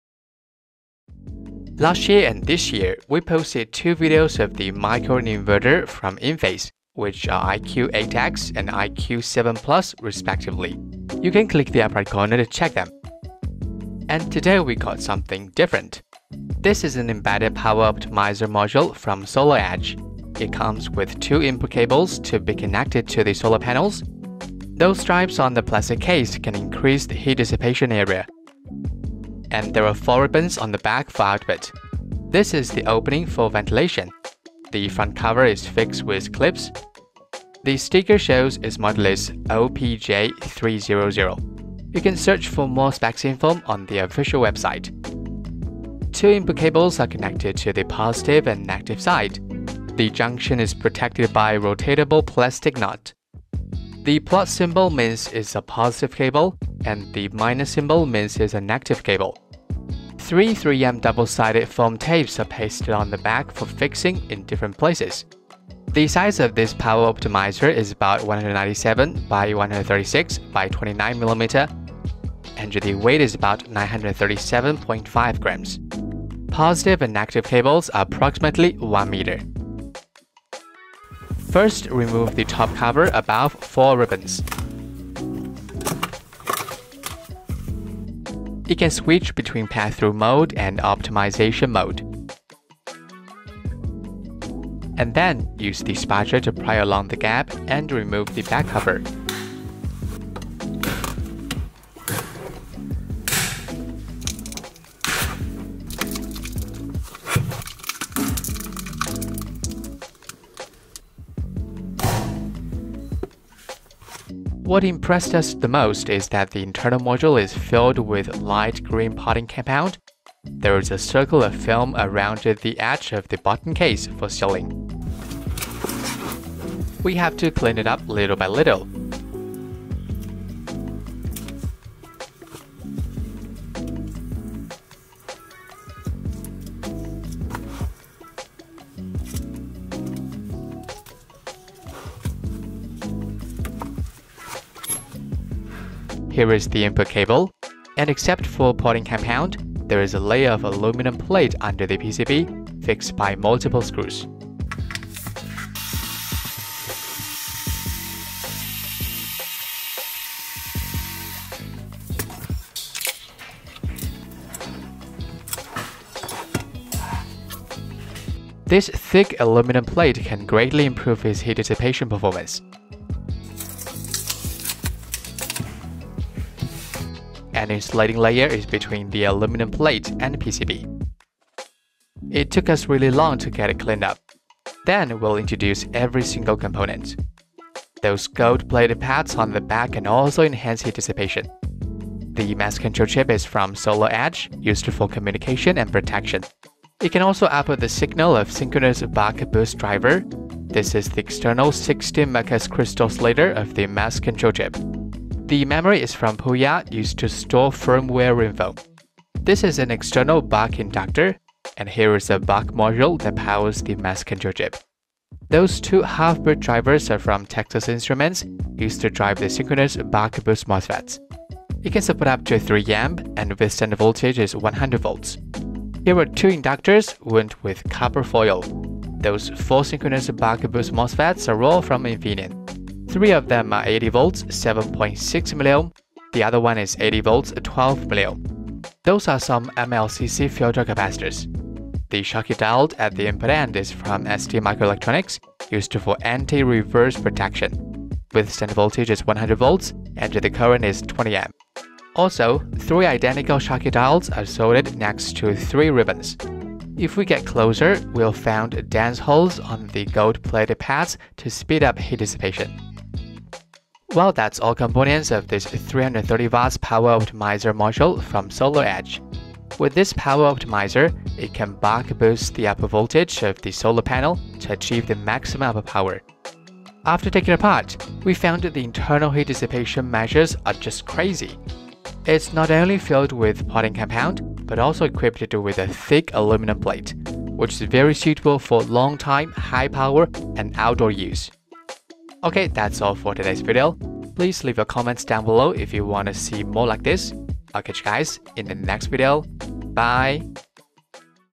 Teardown of SolarEdge 330W Power Optimizer Module Embedded Solution
Introduction
What we gonna take apart today is an embedded power optimizer module from SolarEdge. It comes with two input cables to be connected to the solar panels.
With this power optimizer, it can buck-boost the output voltage of the solar panel to achieve the maximum output power.

Model: OPJ300-LV
Master Control Chip: SolarEdge LV640304AP0
Memory: Puya P25Q40H
Half-bridge Driver: TI LM5101AM
Synchronous Buck-boost MOSFET: Infineon IRF6646, Infineon IRF6668
Schottky Diode: STMicroelectronics STPS20H100CG

Chapter
0:00 Intro
1:34 Teardown
3:02 Analysis
4:24 Summary